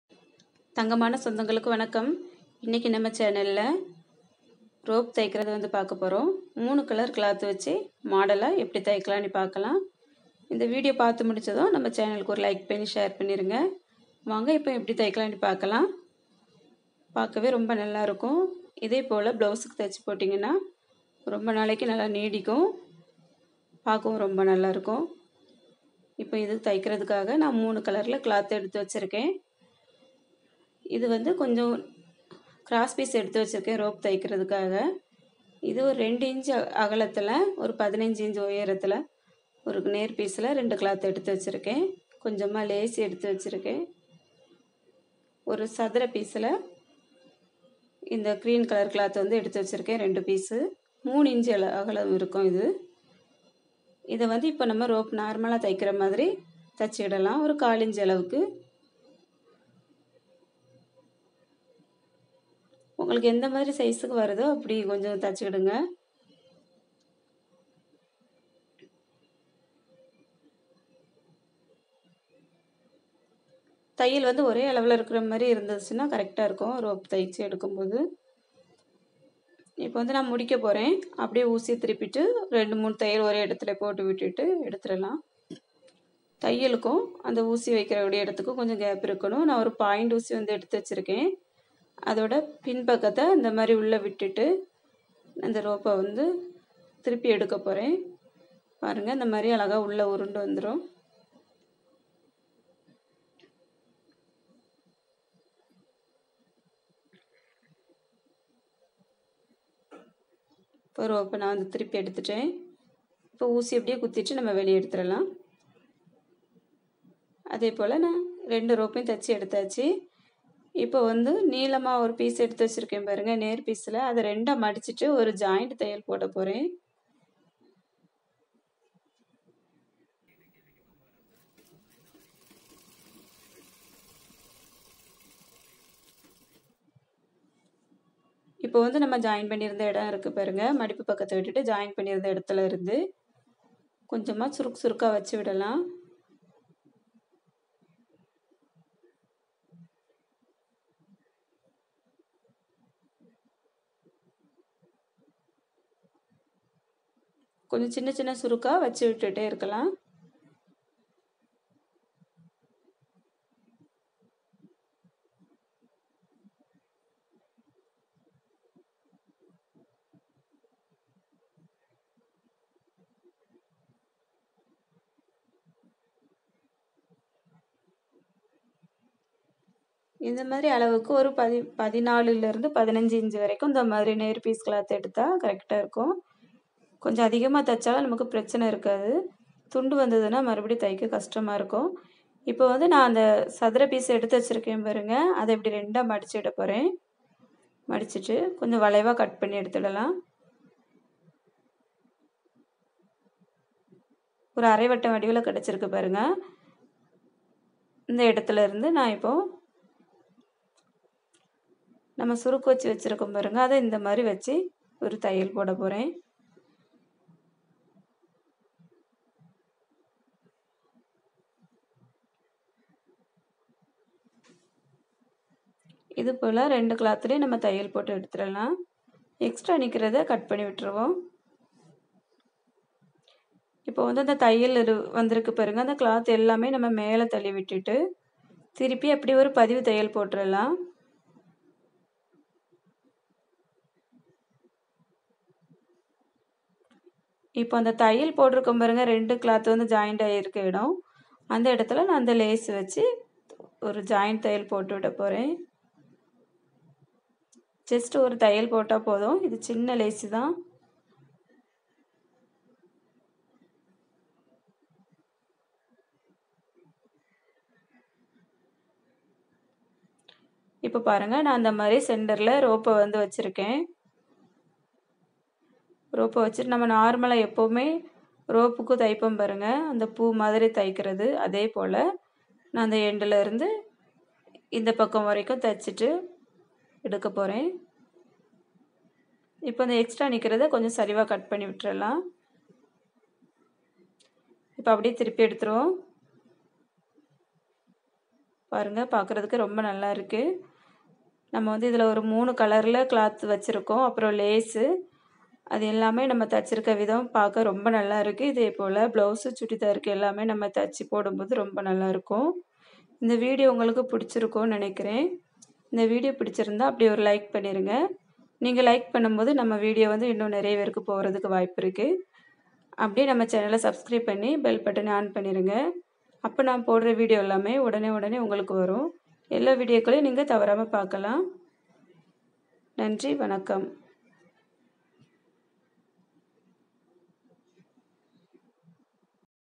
Blouse Bock Rope stitching in tamil/ப்ளவுஸ்க்கு இந்த மாதிரி ரோப் தைத்துப் பாருங்கள் .
பிளவுஸ்க்கு இந்த மாதிரி ரோப் தைத்துப் பாருங்கள் !! அருமையாக இருக்கும்